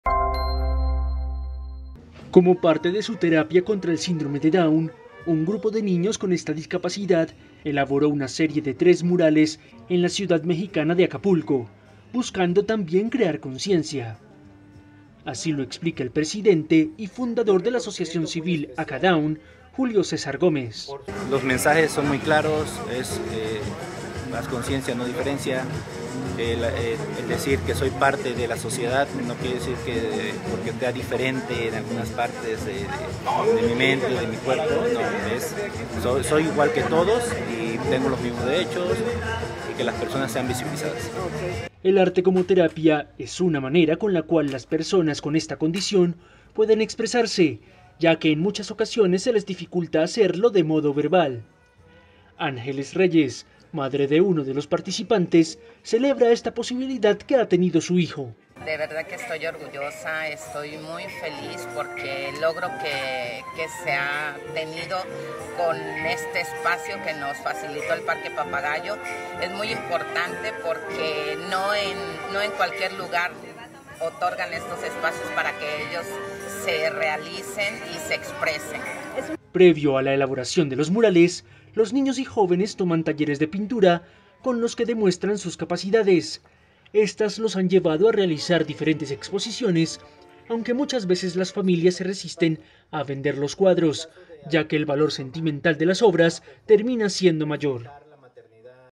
Niños con síndrome de Down en México pintan murales como parte de su terapia
Niños mexicanos con síndrome de Down exhibieron este martes murales pintados por ellos mismos en la ciudad costera de Acapulco para crear consciencia sobre la discapacidad y como parte de una nueva terapia.